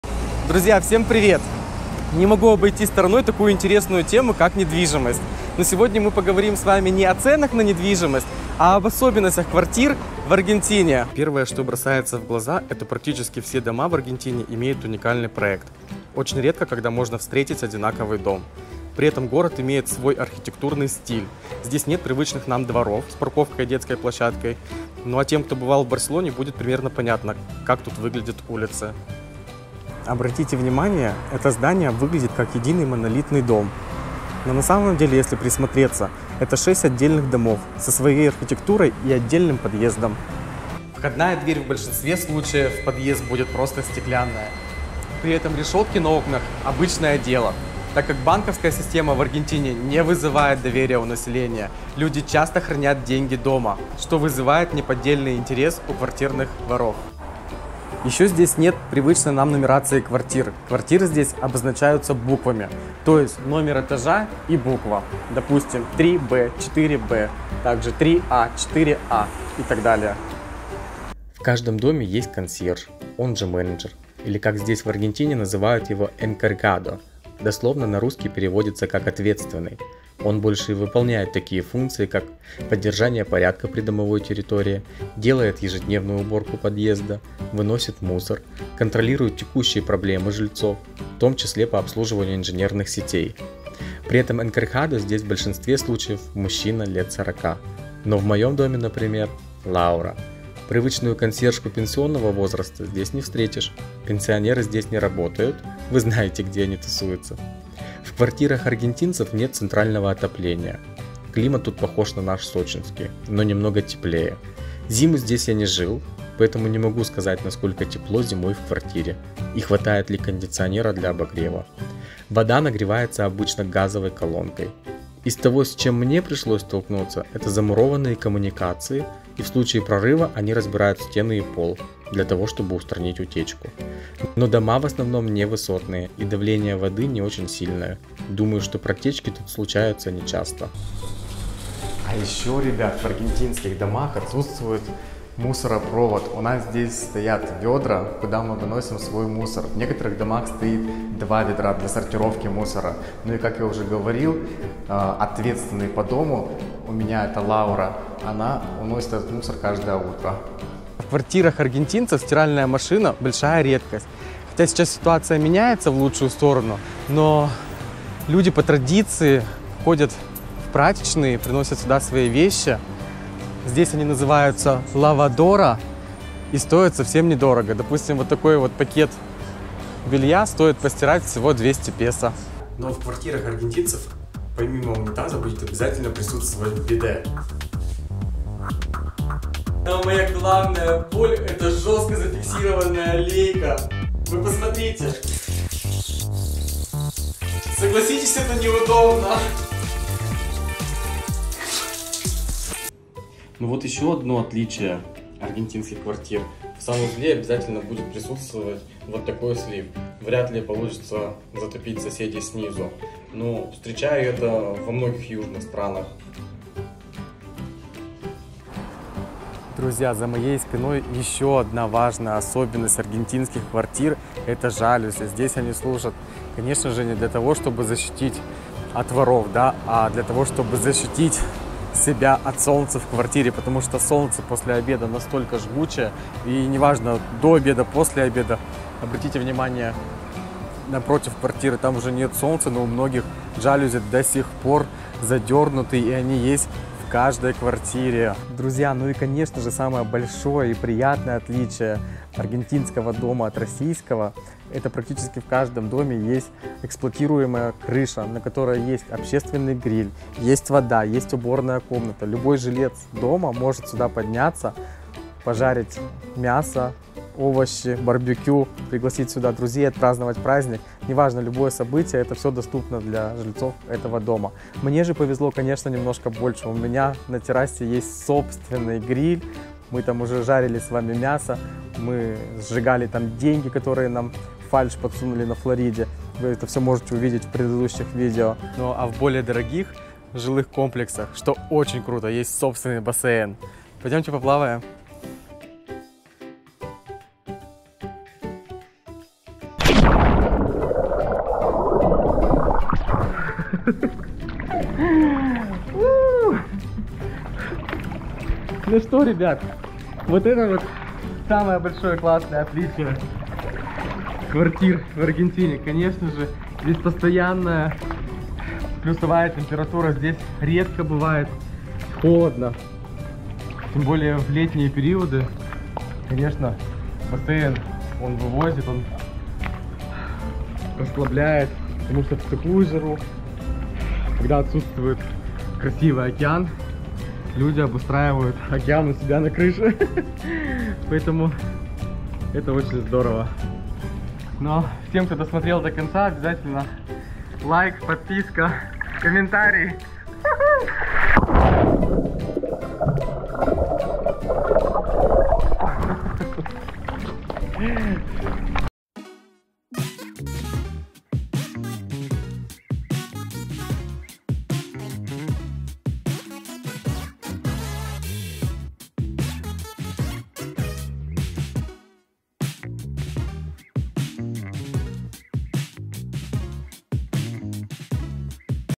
10 СЕРЬЕЗНЫХ ОТЛИЧИЙ КВАРТИРЫ В АРГЕНТИНЕ И РОССИИ
В этом видео я выбрал наиболее интересные особенности квартир в Аргентине, а конкретно в столице Аргентины в Буэнос Айресе! Возможно я учел не все важные детали, пишите в комментариях, что заметили вы!